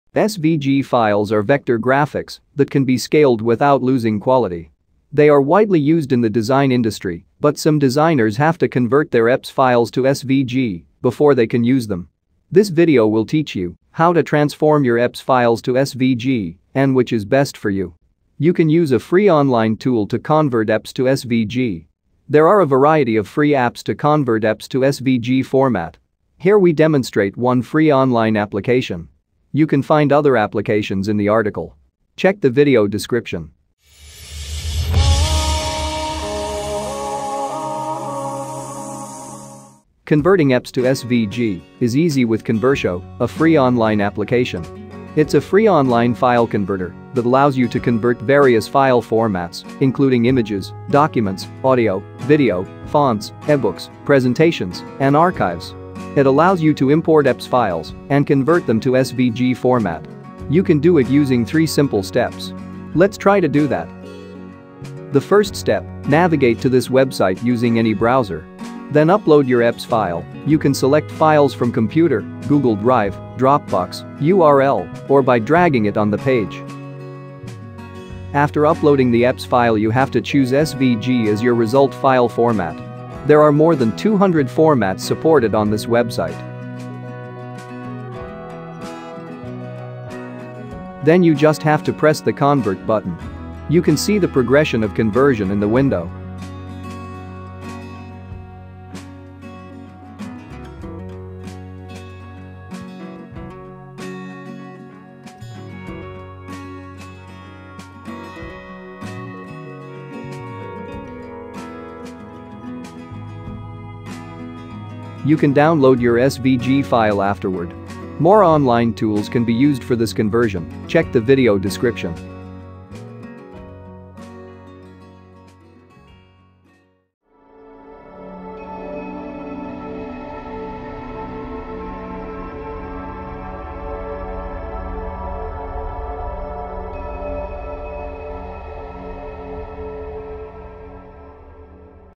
Convert EPS to SVG (10 Online Free Vector Converter Tools)
SVG files are vector graphics that can be scaled without losing quality. They are widely used in the design industry, but some designers have to convert their EPS files to SVG before they can use them. This video will teach you how to transform your EPS files to SVG, and which is best for you.

Timeline:
00:00 Introduction
00:40 EPS to SVG Convert

SPECIAL INVITATION: Join our MineX Tuts Community. Help to other by Sharing your Knowledge.